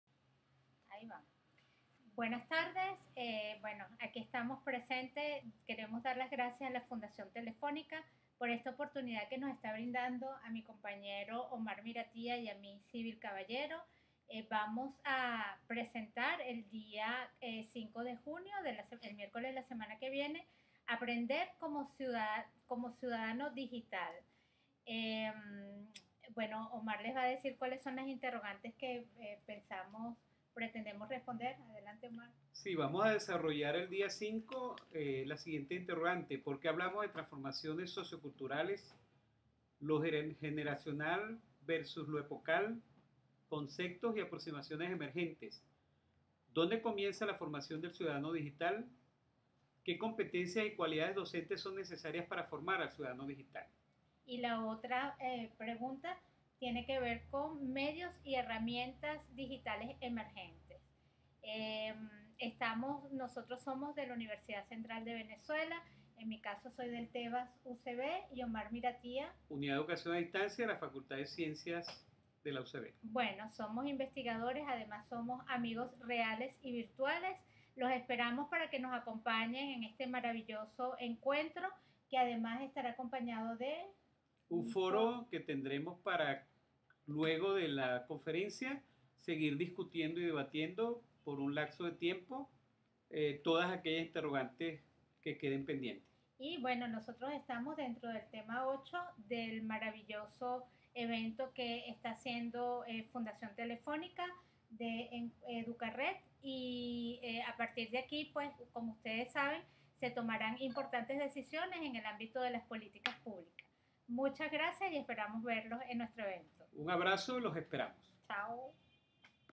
Aprender como ciudadano digital
Video presentación Omar Miratía y Sybil Caballero, en la actividad "Aprender como ciudadano digital" enmarcada en el Tema: Formación permanente. Aprendizaje formal, informal y no formal.